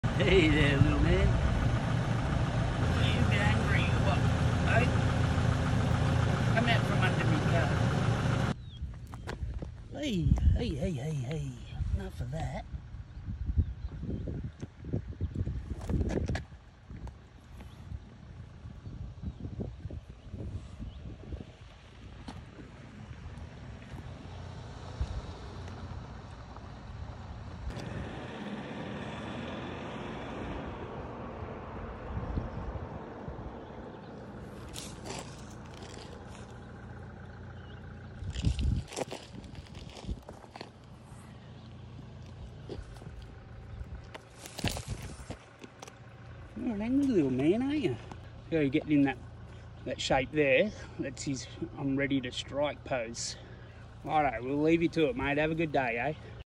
Black Headed Python Snake 🐍 Attack 😡
The black-headed python is a species of snake in the Pythonidae. The species is native to Australia.

Angry 😤 Snake 🐍 Attack ☠️